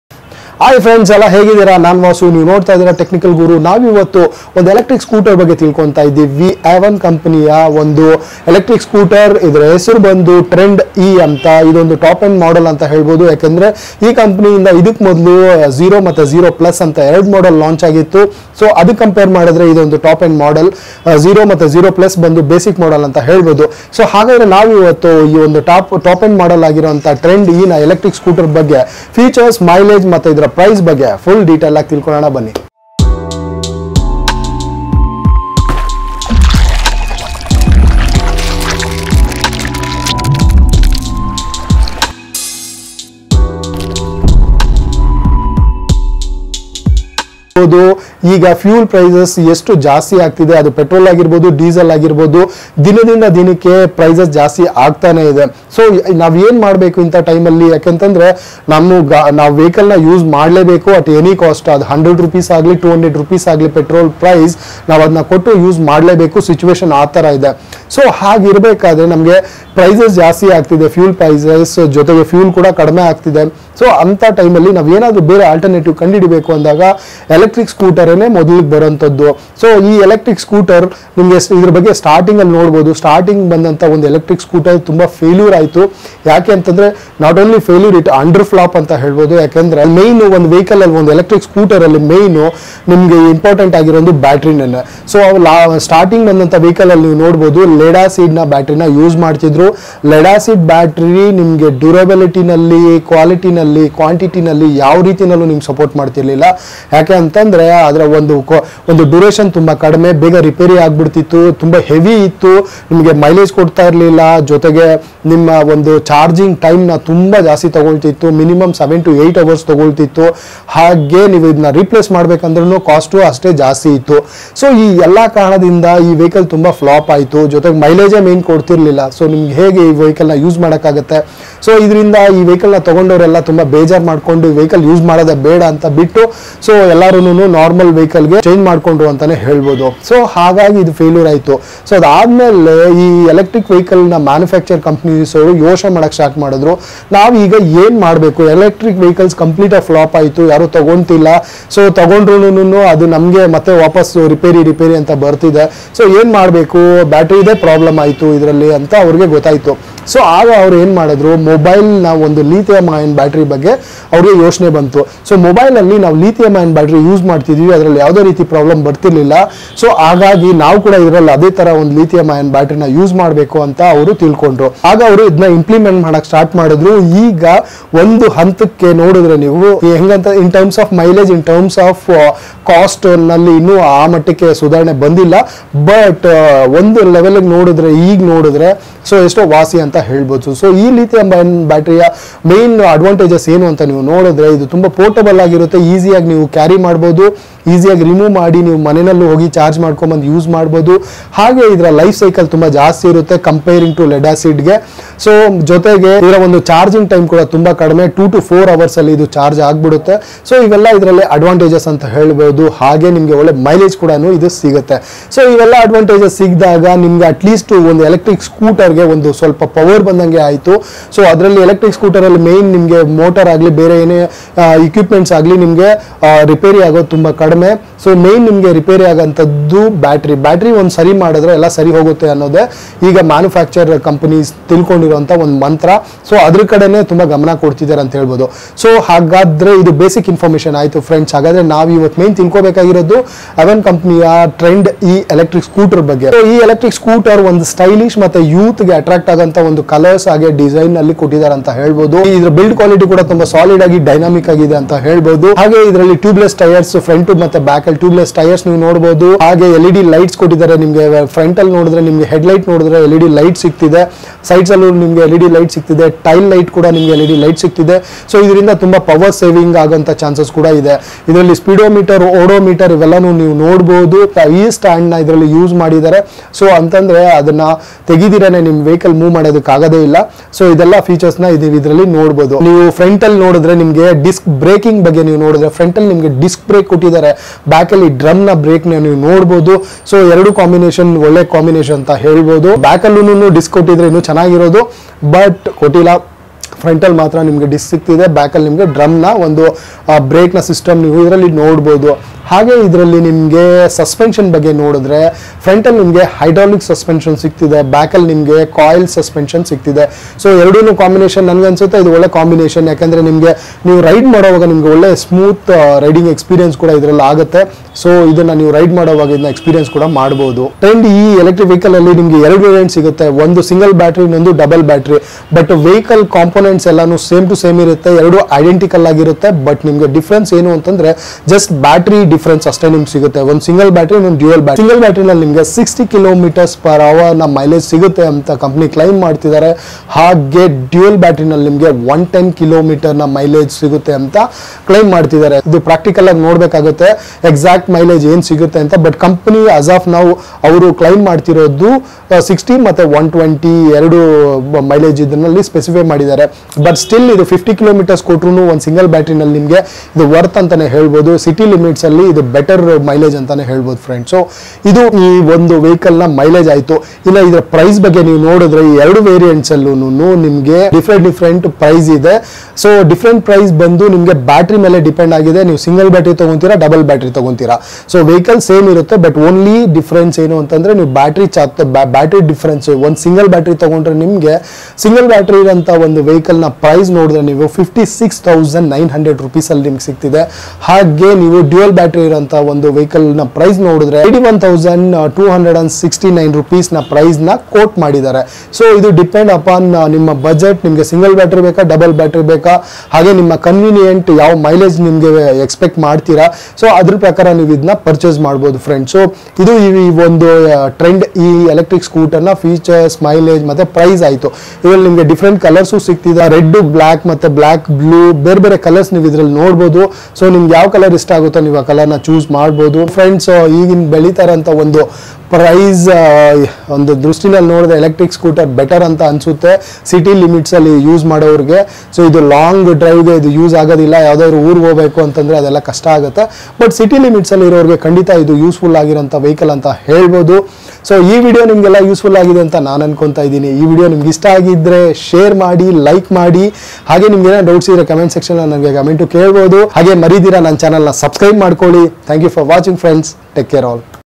Avan Trend E Electric Scooter
Avan Motors India unveiled the Trend E electric scooter at the beginning of this month. And now the company has launched the same at Rs 56,900 (ex-showroom). The company mentions that the high-performance Trend E has an ex-showroom price of Rs 56,900 for the single-battery scooter and Rs 81,269 for the double-battery scooter. The scooter can be booked from today onwards for a token amount of Rs 1,100.